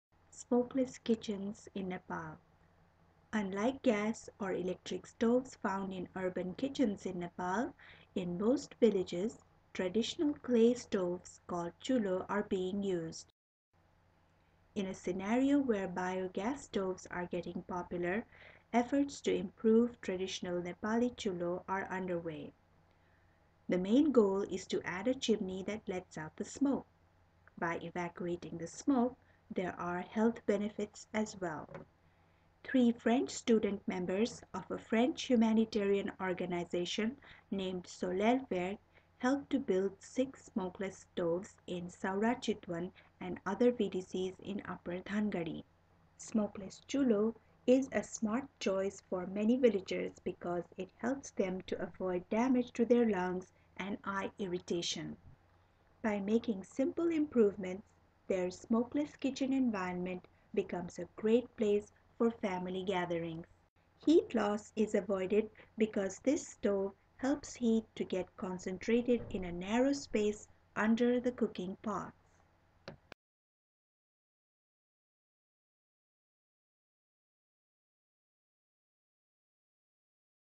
"Chulo"- Smokeless Kitchen In Nepal
"Chulo" Stoves with Chimneys- Smokless kitchens in Nepal
Unlike gas or electric stoves found in urban kitchens in Nepal, in most villages traditional clay stoves called chulo are being used. Timber
 and non- timber forest products are burnt in these stoves for every meal prepaed.

In a scenario where bio gas stoves are getting popular, efforts to improve traditional Nepali chulo are underway.

The main goal is to add a chimney that lets out the smoke.
By evacuating the smoke there are health benefits as well.

3 French student members of a French Humanitarian organization named Soleil Vert helped to build 6 smokeless stoves in Sauraha Chitwan and other VDCs in upper Dhangadi.

Smokeless Chulo is a smart choice for many villagers because it helps them avoid damage to their lungs and eye irritation. By making simple improvements their smokeless kitchen becomes a great place for family gatherings. Heat loss is avoided because this stove helps heat to get concentrated in a narrow space under the cooking pots.